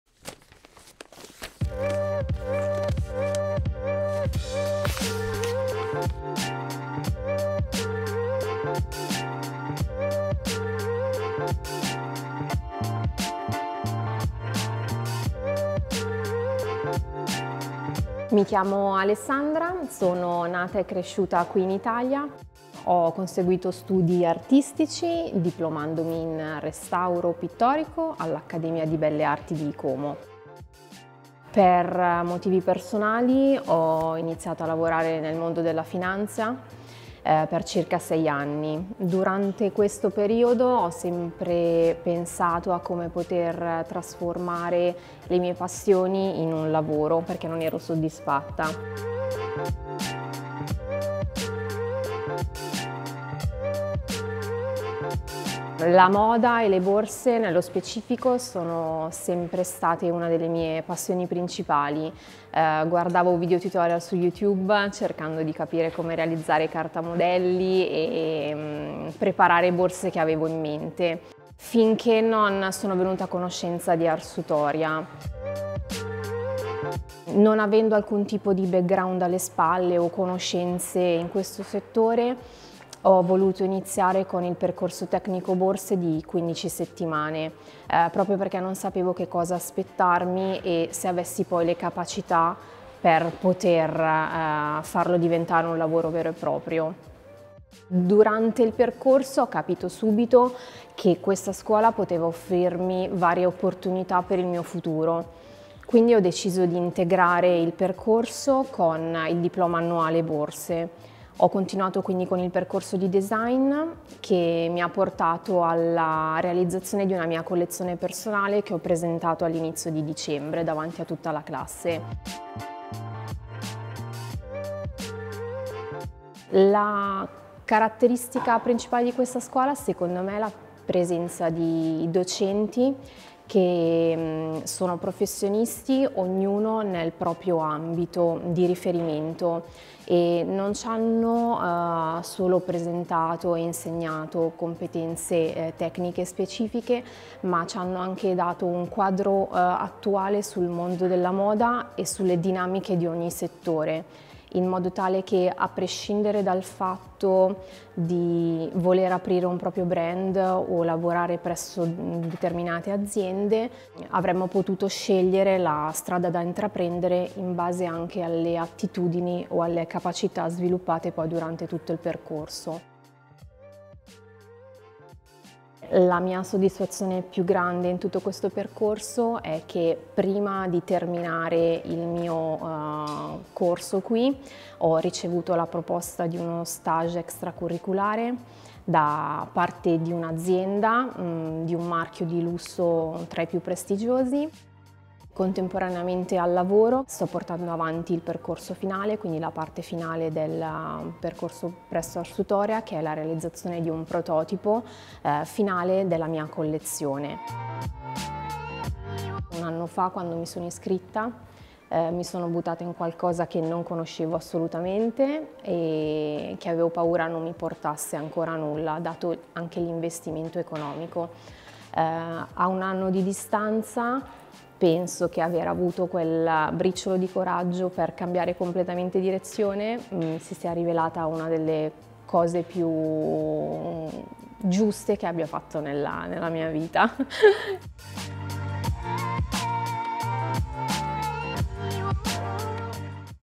Student Testimonial: Alessandra Colombo, Bag 1-Year Diploma Student
Alessandra Colombo, Bag 1-Year Diploma student, tells her experience from the day she took the courage to change her life path, starting the course in Arsutoria, until today, when, even before the end of the course, she was offered to work for one of the most important luxury fashion brands.